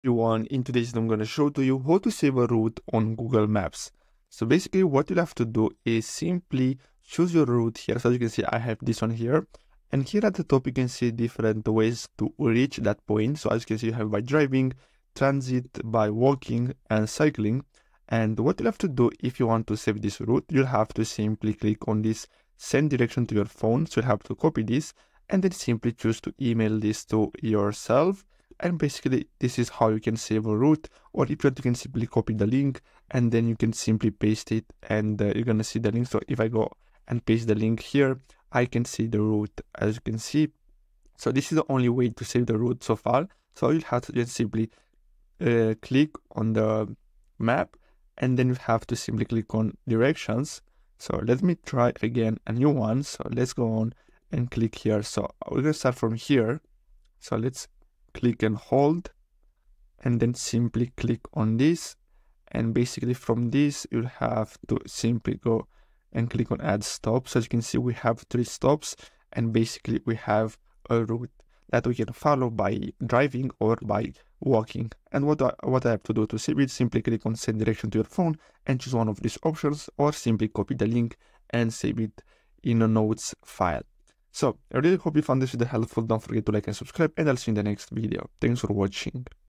How To Save A Route On Google Maps (2026)
Saving a route on Google Maps is a simple and efficient way to ensure you have easy access to directions for future reference. To save a route, begin by entering your starting point and destination in the search bar. Once the route is displayed, click the three dots in the top right corner of the screen and select "Save route". You will then be prompted to name the route and choose a folder to save it in for easy organization.

By saving routes on Google Maps, you can quickly access directions without the need for repeated searches, saving you time and hassle in the long run.